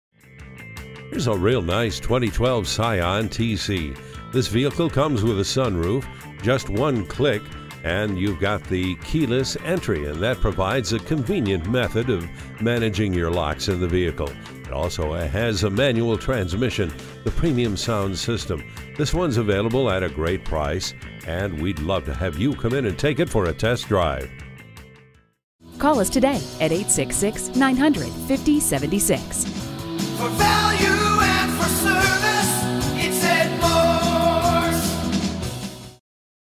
2012 Scion tC Tampa FL St. Petersburg, FL 169005A - SOLD
Year: 2012
Make: Scion
Model: tC
Trim: 2dr HB Auto
Engine: 2.5l 4 Cyl. Fuel Injected
Transmission: FWD Manual
Color: Gray
Interior: Dark Charcoal
Mileage: 13642
Stock #: 169005A
Don't let this 2012 Scion tC 2dr HB Auto get away! This one's available at the low price of $16,993. This vehicle comes with a sunroof. With just one click, keyless entry provides a convenient method for managing your locks. According to a review from CarsDirect, At the end of the day, the tC ends up being something of a fashion statement, and I'm actually OK with that. Looking for a specific model? Call today for more information.

Address:
101 East Fletcher Avenue